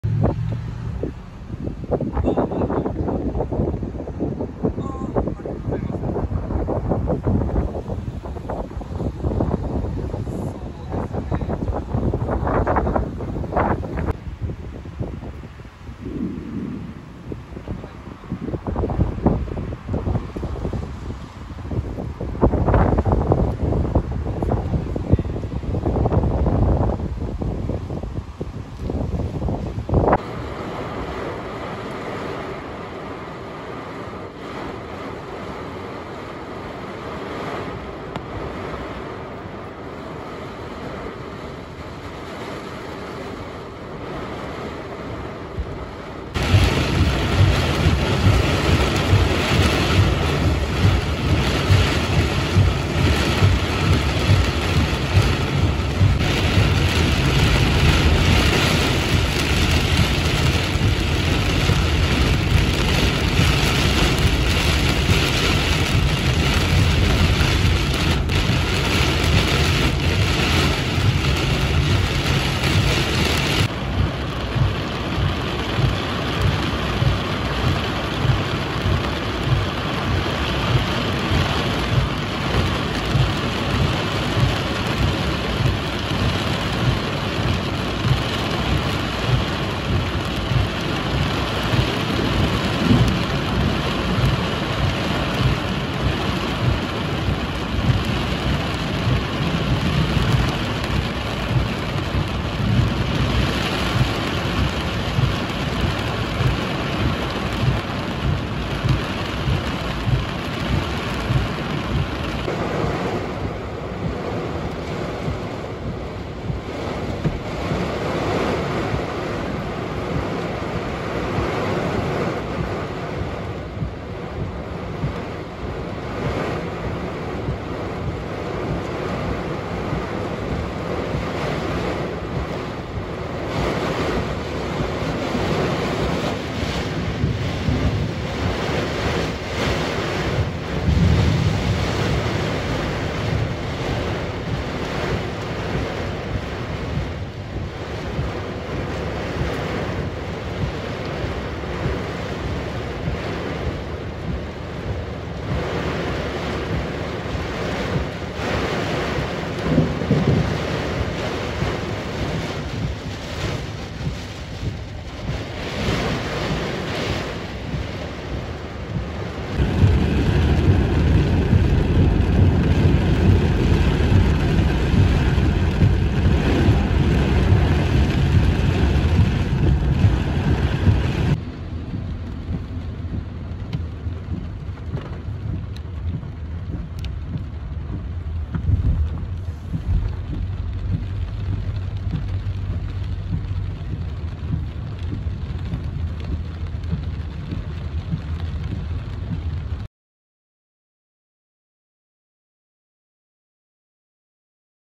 愛知県西尾市室町撮影(ゲリラ豪雨)
徐々に黒い雲が近づいてきて、急に突風が。そして一気に激しい雨が。前も見えないほどの豪雨でした。